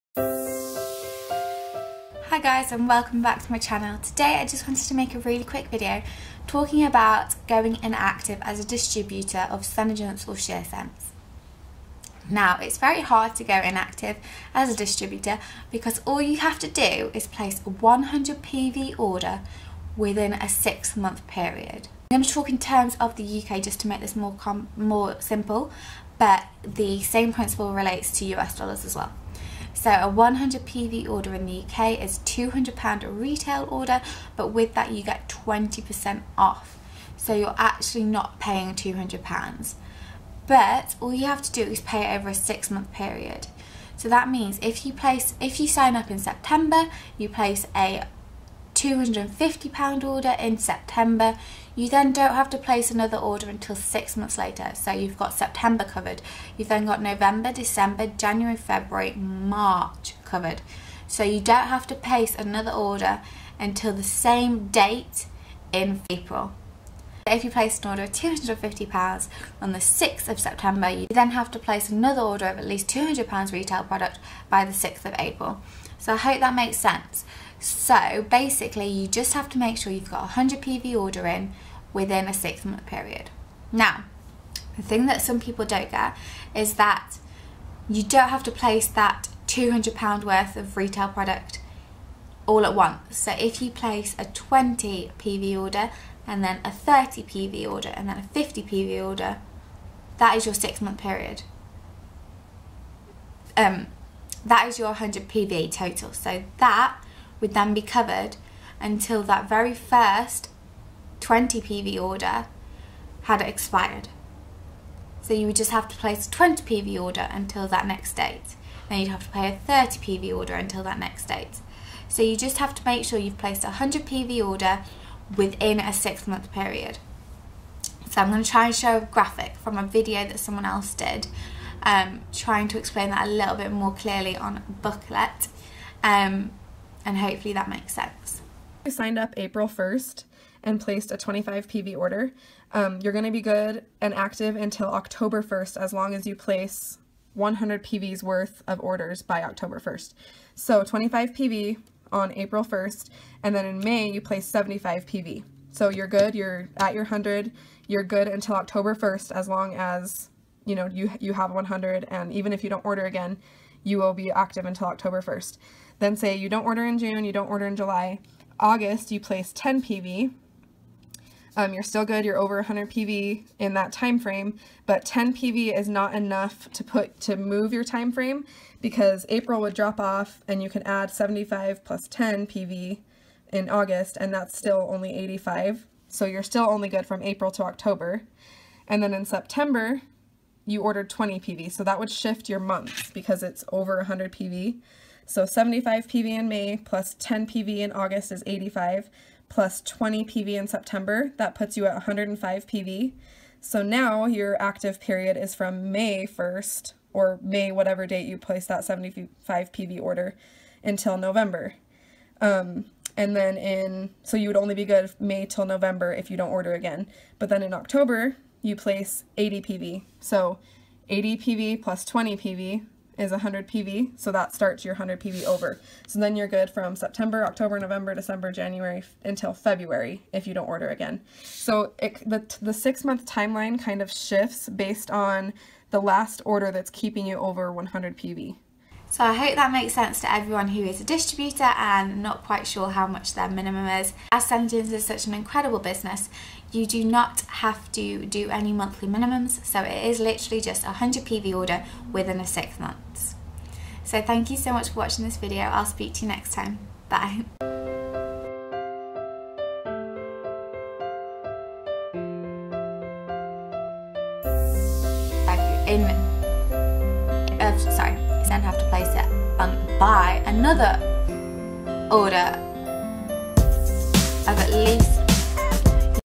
Going Inactive as a Senegence Distributor!?
It's really hard to go inactive as a Senegence Distributor. Here's how to remain active!